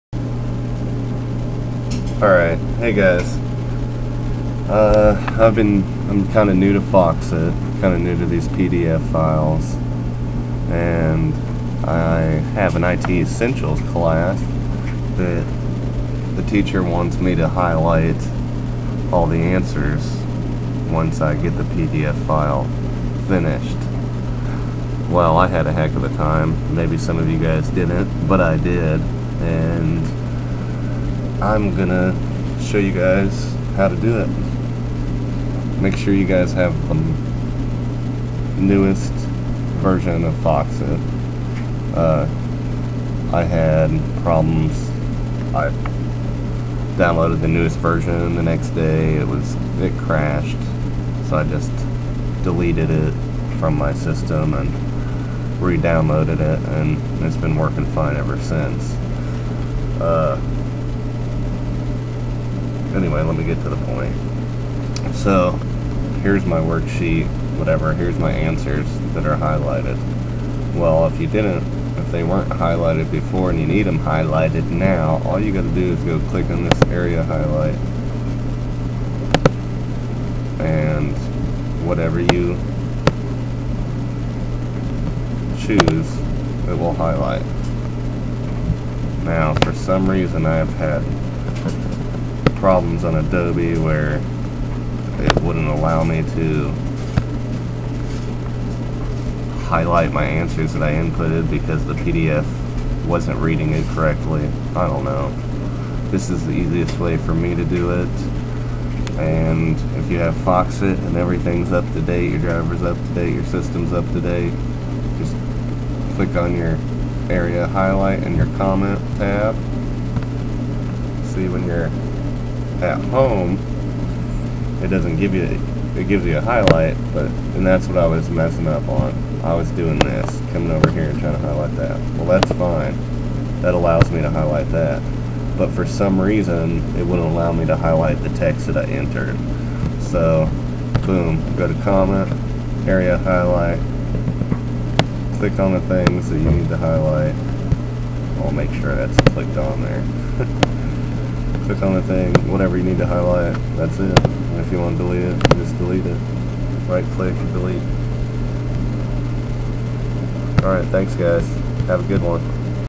Highlighting Text in a PDF File using Foxit (Free Version) (Circa 2013)
This is just a simple walk through of how you can highlight text you have entered on a PDF File. This is using the latest version (Fall of 2013) of the Free Version of Foxit. You can find the download @ I know it's simple stuff but when you are first introduced to all these things it can be a little overwhelming. I am no expert, I just thought this may help a few people out there.

(Update) This is a twelve year old video at this point and still seems to be helping a few people every now and again.

Send me a thanks, hello, or an item that may get featured in a future video:

VintageRetro
Shelbina, MO. 63468